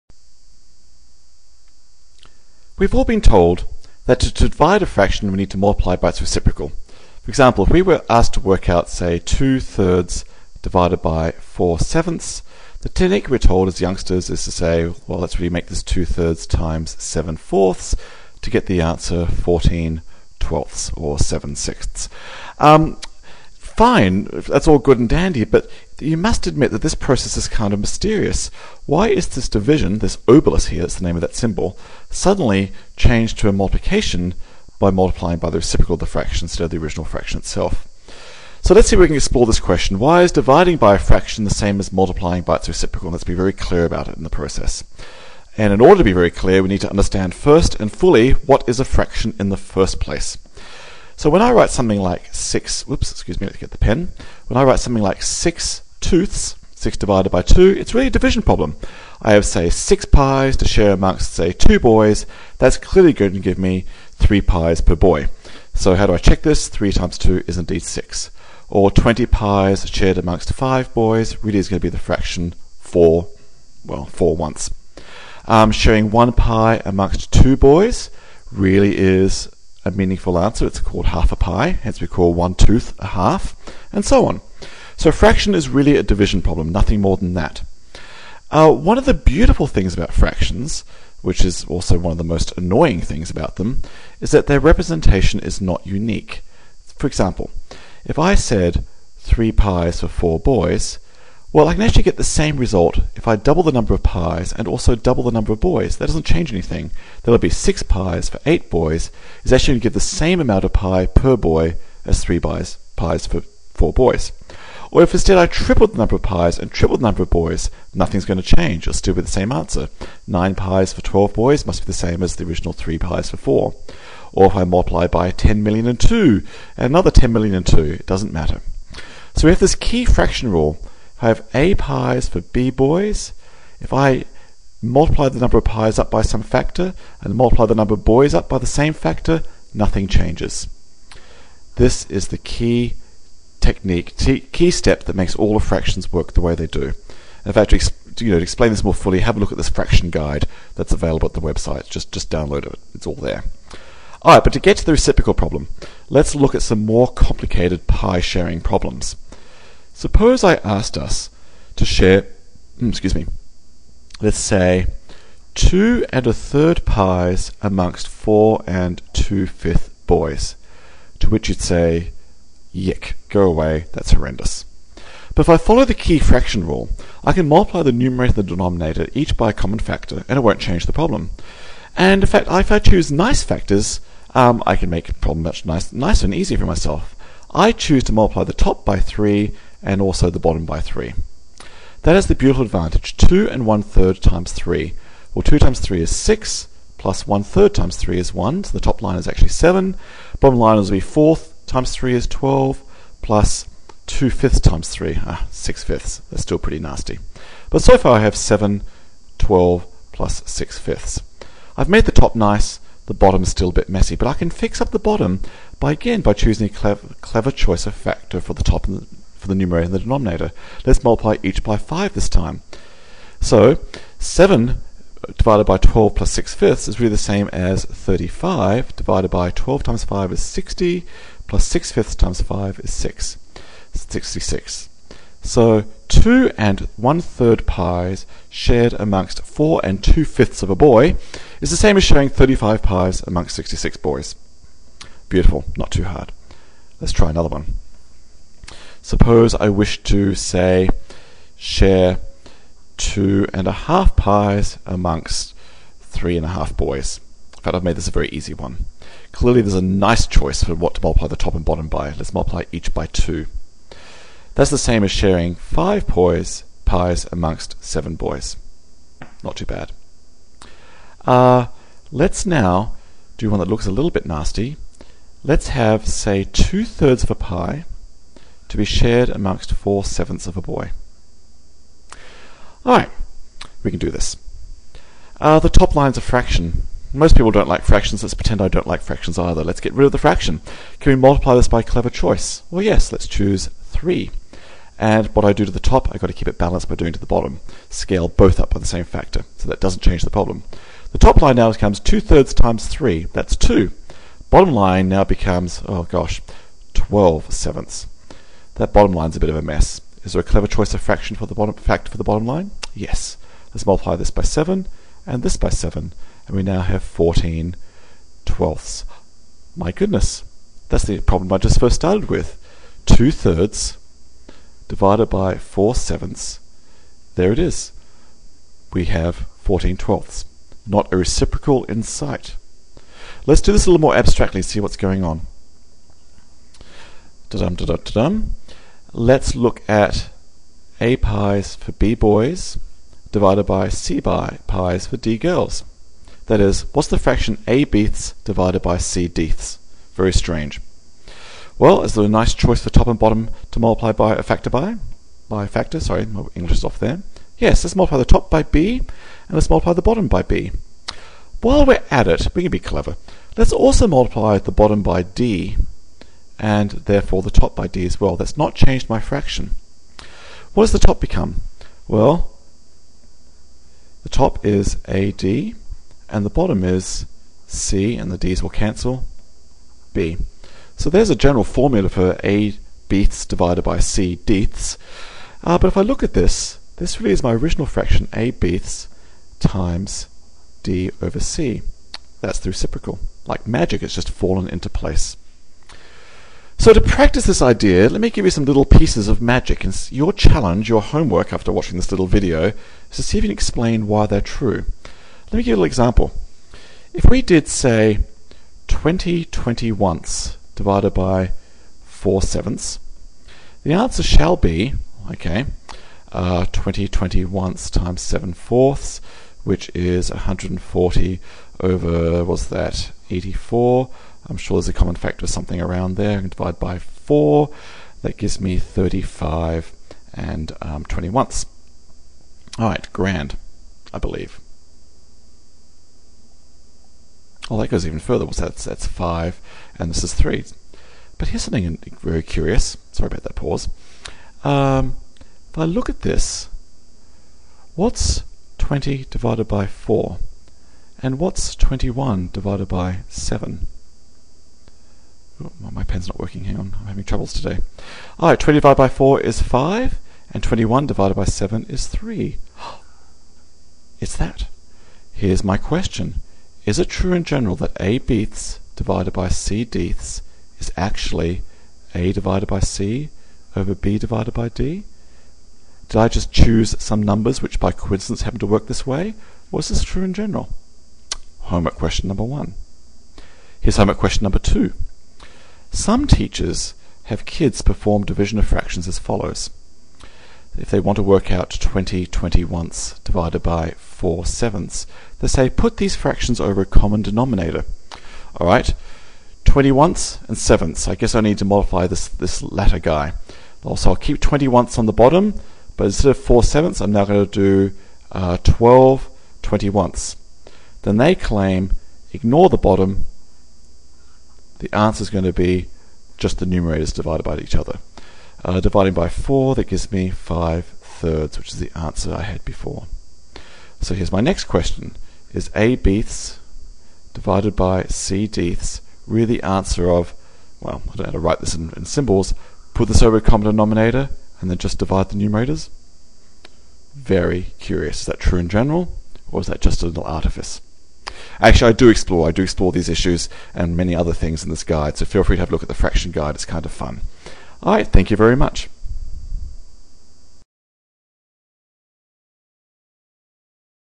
Why is dividing by a fraction the same as multiplying by the reciprocal? (TANTON Mathematics)
Is it at all obvious that dividing by 4/7 is the same as multiplying by 7/4? We are all told to do this. Is it true? Let's find out!